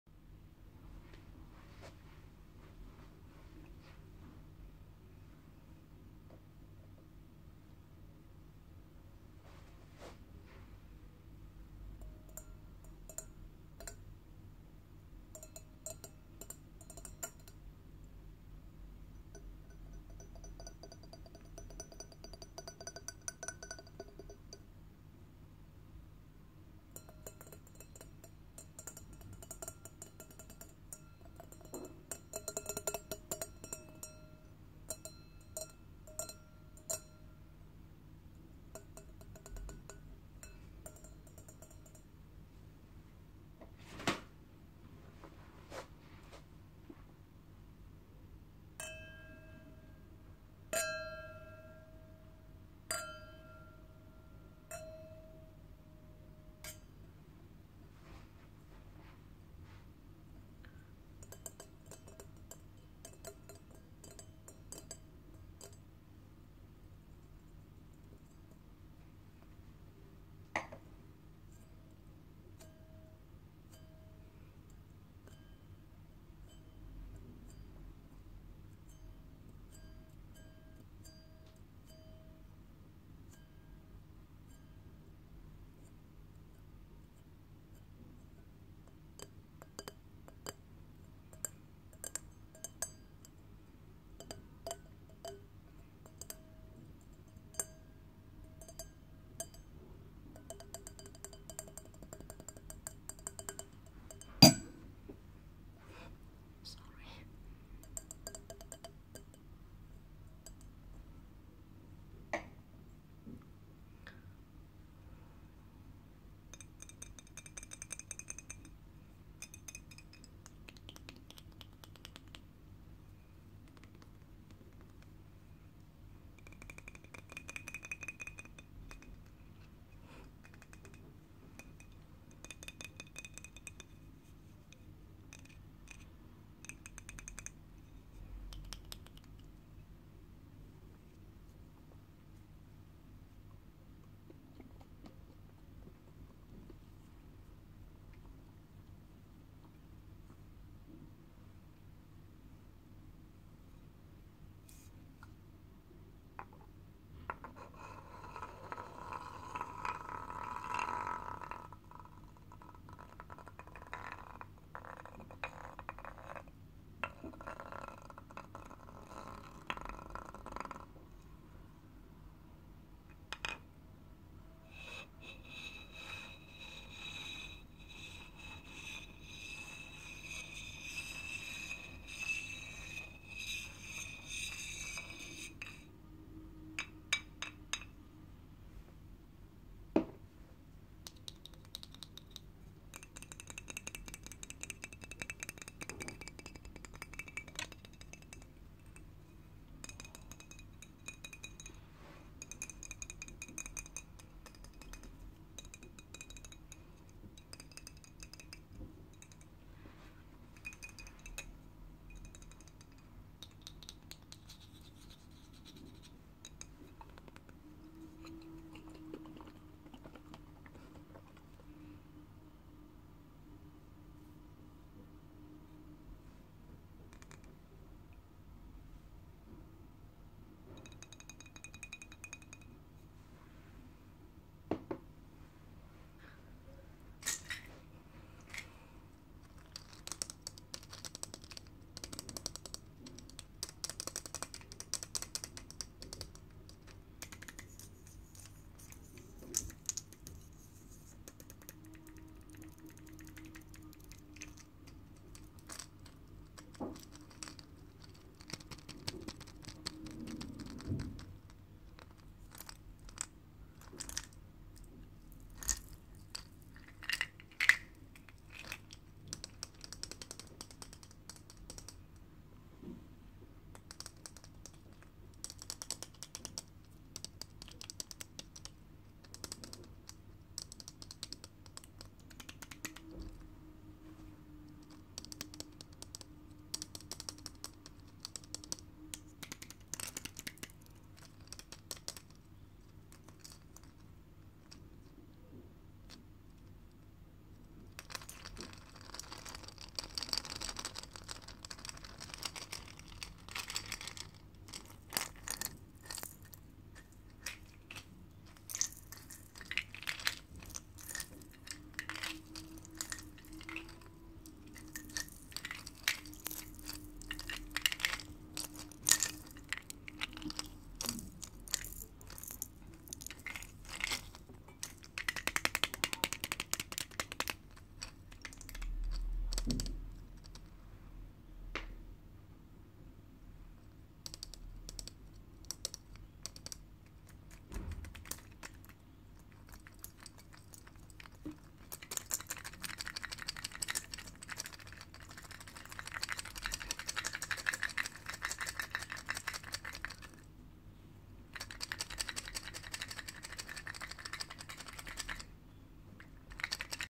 🌟 Tap That Glass 🌟 Pure Varied Fast & Slow Nail Tapping (& Scratching) / ASMR ♡ Part 1
Hey everyone! ♥

Please use earphones ★

Let me know what videos you want to see next in the comments below♡

♥ Victoria Luz ♥